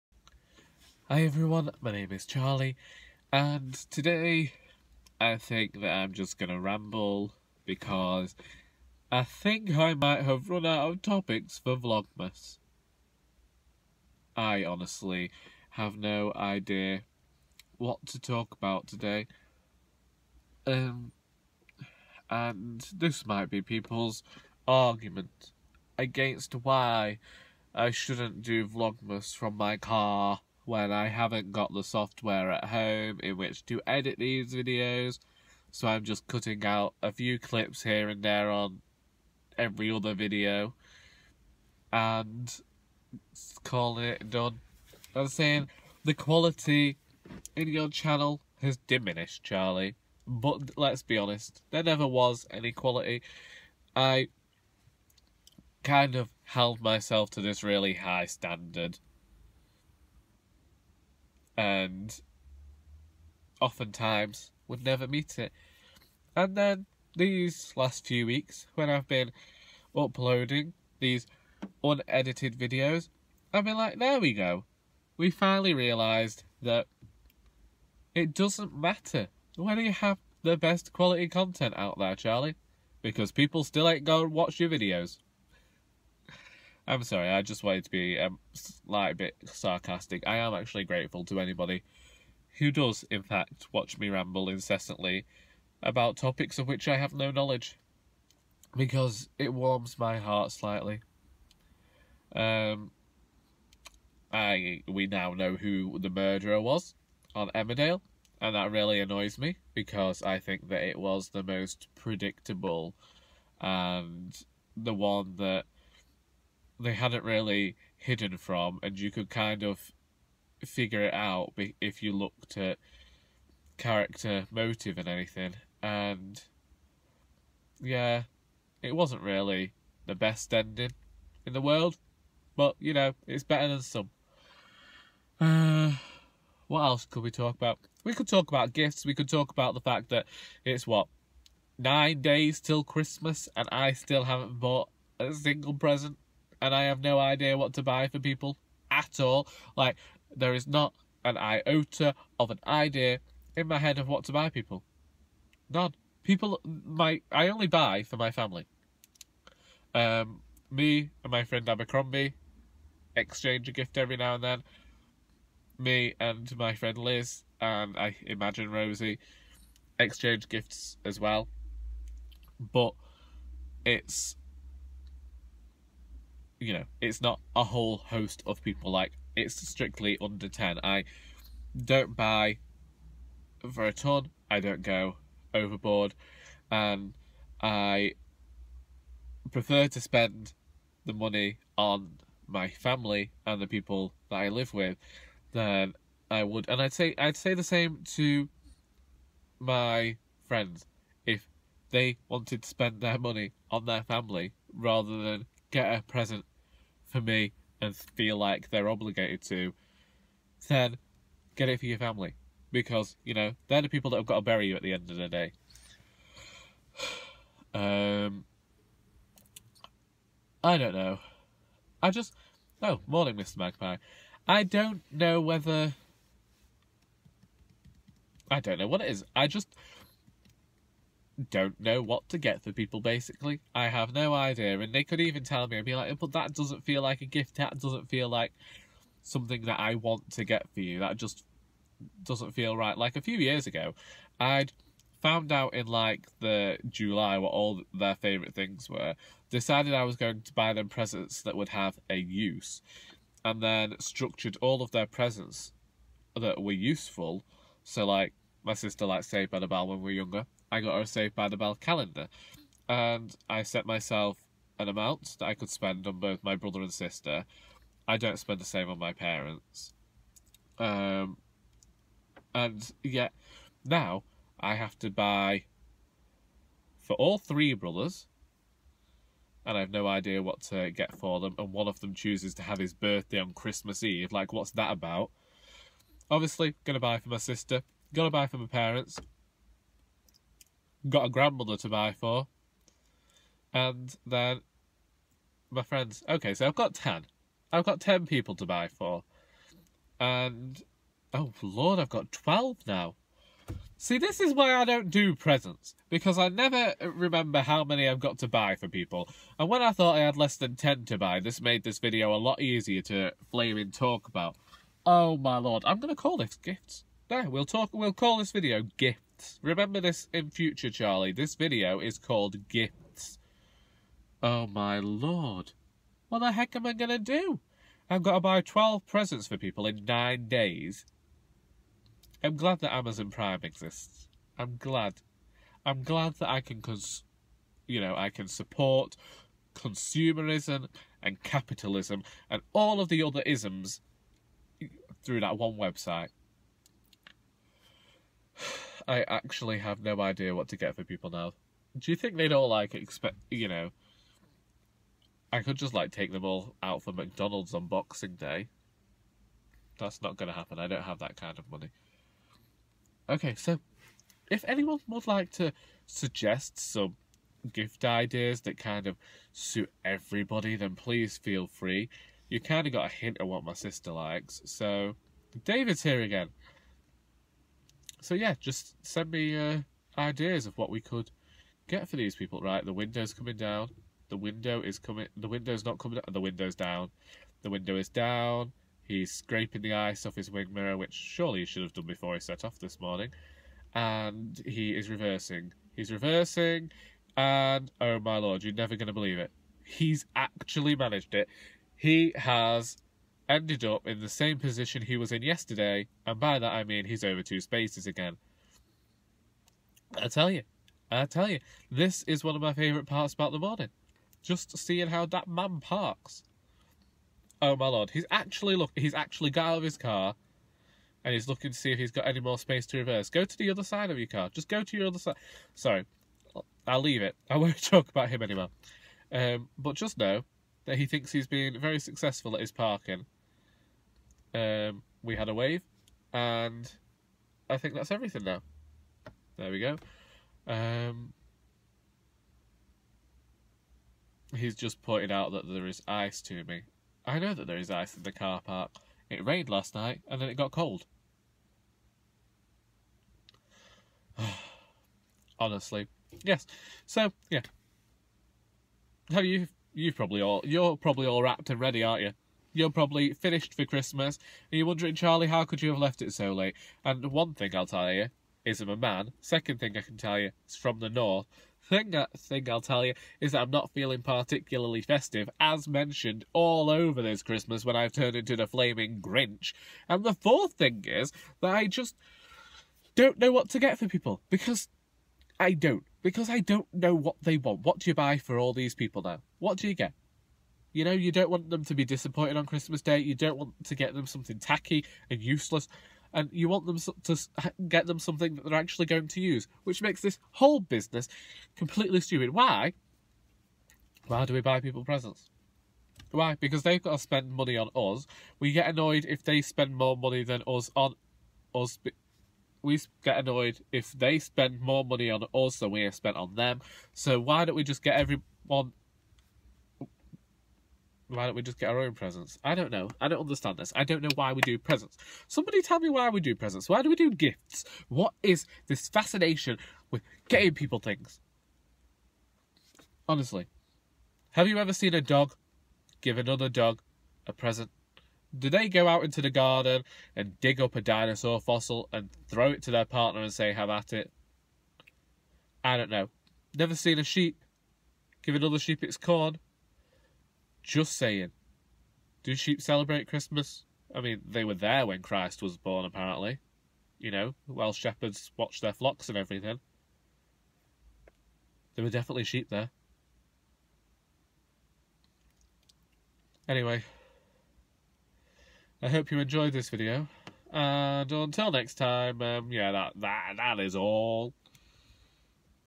Gifts | Vlogmas Day Sixteen
In which I ramble about gifts.

Goodreads: Charles Heathcote